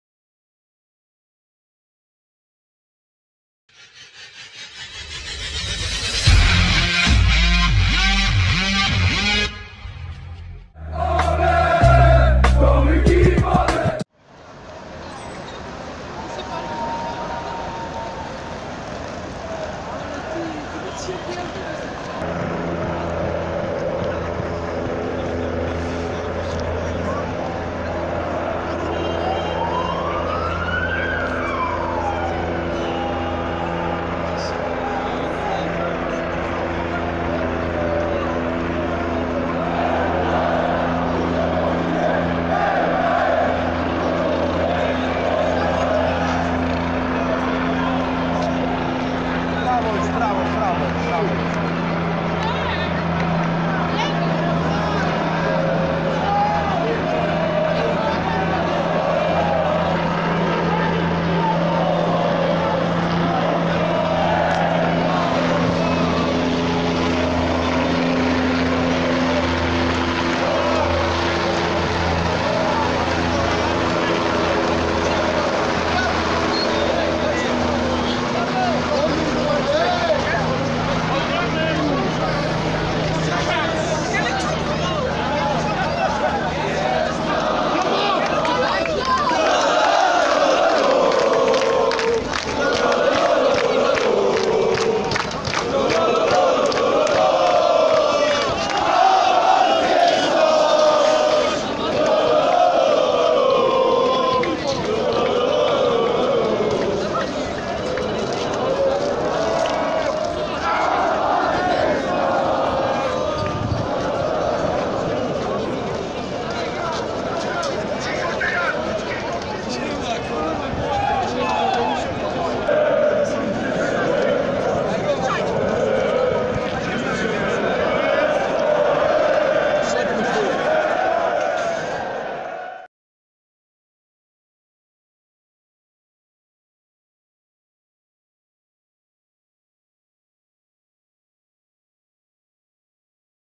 Przemarsz Kibiców ŁKS-u przez miasto po awansie do Ekstraklasy
JEŚLI JESZCZE NAS NIE SUBKRYBUJESZ ZRÓB TO !!!